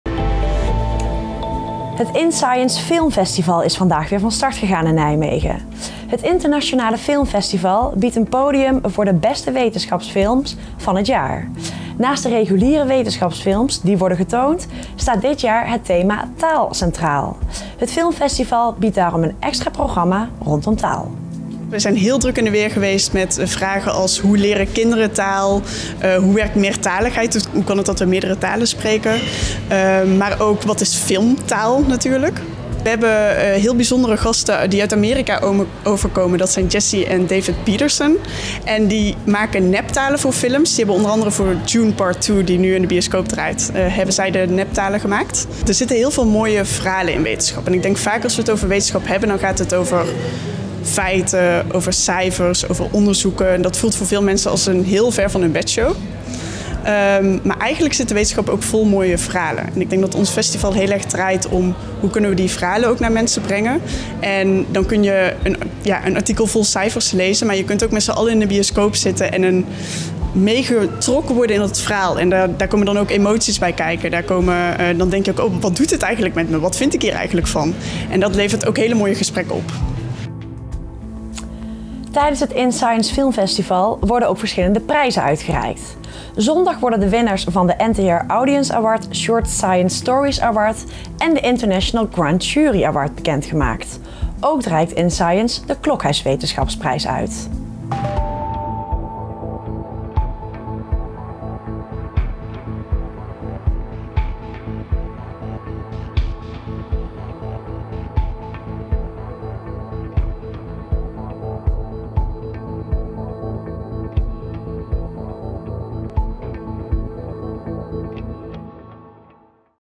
InScience strijkt weer neer in Nijmegen || RN7 REGIONIEUWS
Het InScience filmfestival is vandaag weer van start gegaan. Het internationale filmfestival biedt een podium voor de beste wetenschapsfilms van het jaar. Naast de reguliere wetenschapsfilms die worden getoond, staat dit jaar het thema ‘Taal’ centraal. Het filmfestival biedt daarom een extra programma rondom taal.